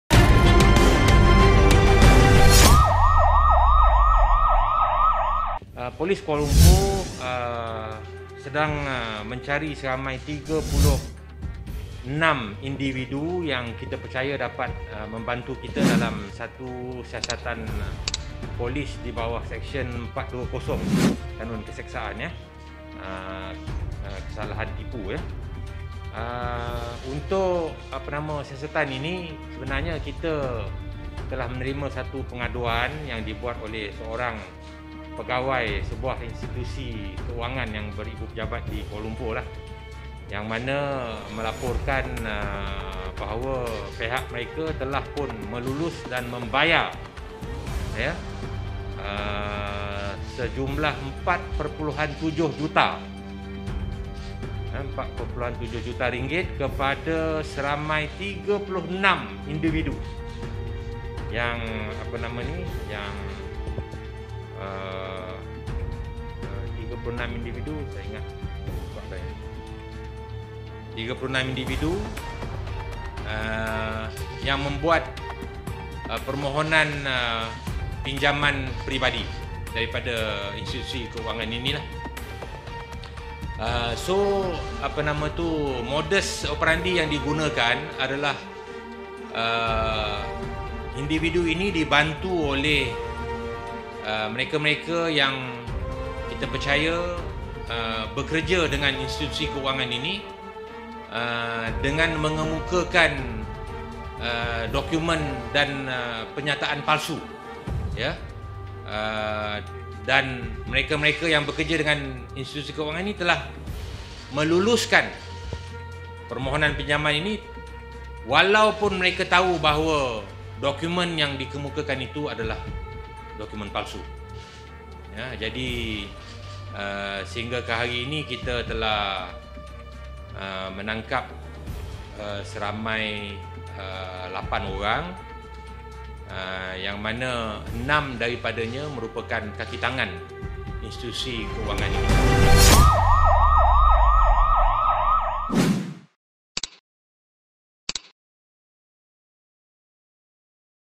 MKL Crimedesk | 8 Individu Sindiket Tipu Pinjaman Peribadi Termasuk 6 Pegawai Bank
mensasarkan individu yang tidak layak memohon pinjaman bank yang disenarai hitam dan tidak mempunyai pekerjaan tetap.

sindiket turut menggunakan nama mangsa dan dokumen palsu untuk buat pinjaman peribadi.

bagaimanapun, kegiatan licik sindiket penipuan pinjaman peribadi ini tumpas apabila 8 individu termasuk 6 daripadanya kakitangan sebuah institusi perbankan ditahan.

kesemua suspek ditahan pada 1 mei lepas di tiga lokasi berlainan di lembah klang.

operasi dilakukan jabatan siasatan jenayah komersial ipk (ibu pejabat kontijen) hasil daripada laporan seorang pegawai institusi kewangan bahawa pihaknya telah melulus dan membayar rm4.7 juta kepada 34 individu yang membuat permohonan pinjaman peribadi itu.

ketua polis kuala lumpur, komisioner datuk azmi abu kassim berkata, ekoran itu polis memburu 34 individu bagi membantu  siasatan berhubung sindiket penipuan pinjaman peribadi di sebuah institusi kewangan di sini dengan menggunakan dokumen palsu.

dua suspek telah tampil untuk membantu siasatan sebelum ini.

siasatan mendapati kakitangan di institusi perbankan tersebut membantu 34 individu terbabit yang berumur antara 24 hingga 45 tahun itu mengemukakan dokumen dan penyataan palsu untuk mendapatkan kelulusan pinjaman.

suspek yang diburu itu dipercayai menyediakan dokumen sokongan untuk bakal peminjam seperti surat lantikan kerja dan penyata kewangan palsu bagi melayakkan peminjam mendapat pembiayaan .

jelasnya, peminjam berkenaan hanya menerima 30 peratus daripada jumlah pembiayaan yang diluluskan bank itu.

dan bakinya akan disimpin oleh sindiket termasuk pegawai bank dengan alasan mereka akan membayar ansuran bulanan selama 20 bulan dan selebihnya adalah komisyen atas kerja yang mereka telah lakukan.

polis juga mendapati, purata setiap seorang daripada 34 suspek ini diluluskan pinjaman berjumlah rm150,000 dan ke atas.

orang ramai yang mempunyai maklumat atau pertanyaan diminta menghubungi ketua bahagian siasatan jenayah komersial ibu pejabat polis daerah putrajaya, asisten superintendan mohamed hanafiah mohamed nawi di talian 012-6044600 atau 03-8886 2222.

kes disiasat di bawah seksyen 420 kanun keseksaan kerana menipu.